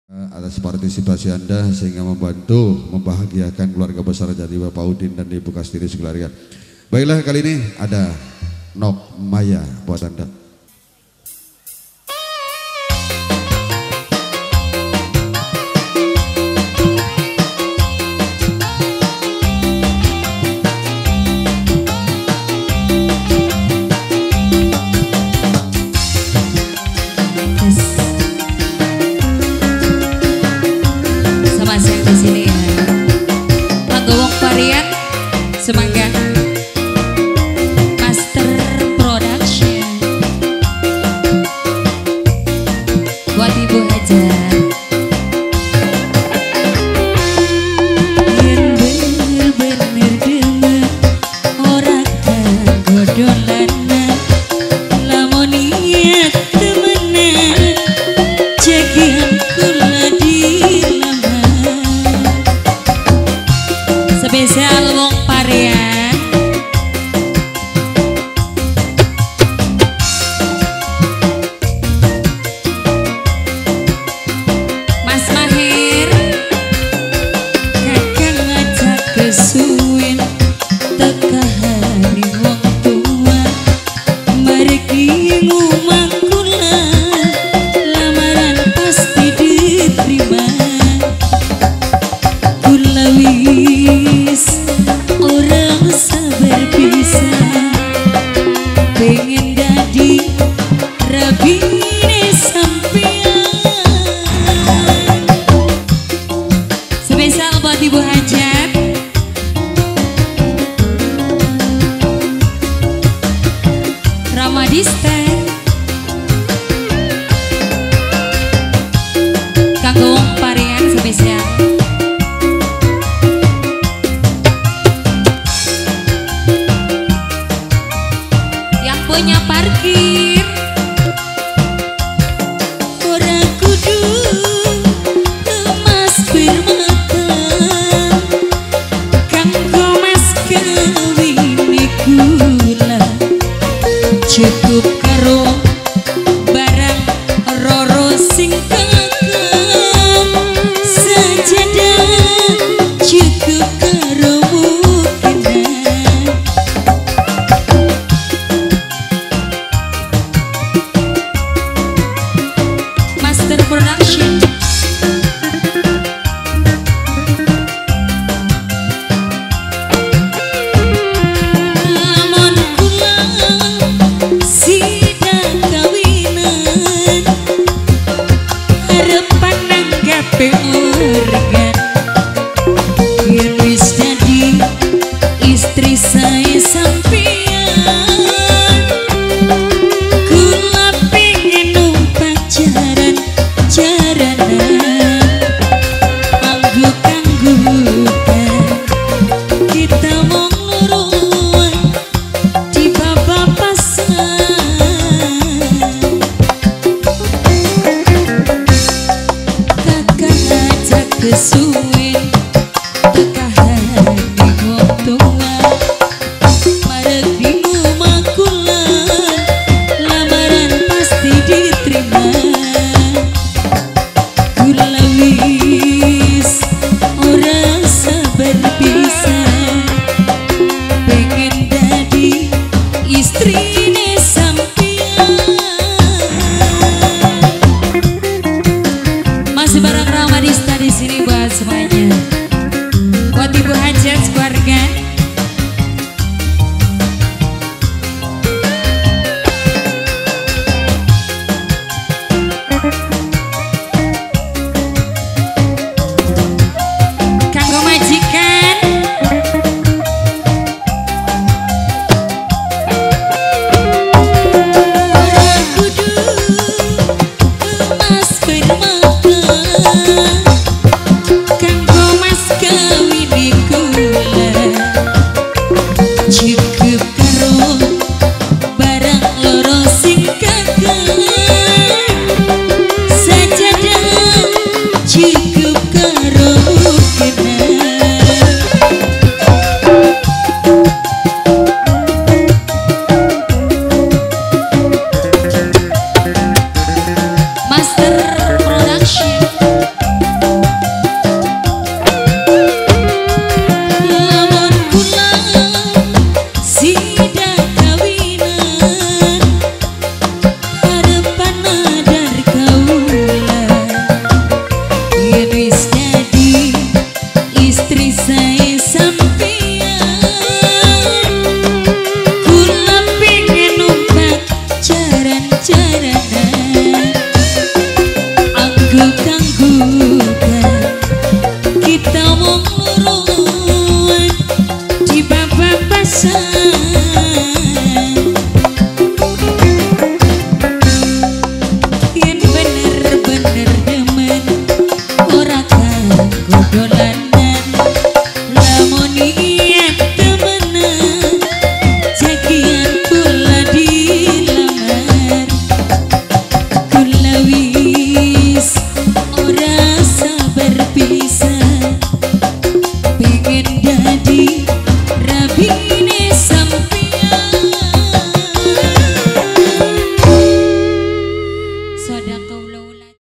RAMADISTA (Jaran-Jaranan) Kroya,30 Maret 2022.
Dok.Hajat :
Bpk.KAERUDIN (Udin) - KASTINI
Pernikahan :
TIRTA dengan NOVI
Rasulan :
FUJI ELIYANTI
Khitanan :
FADIL ENDI JUNIOR
Pelaksanaan :
RABU,30 MARET 2022
Alamat :
Blok PORTAL Desa KROYA Kecamatan KROYA - INDRAMAYU.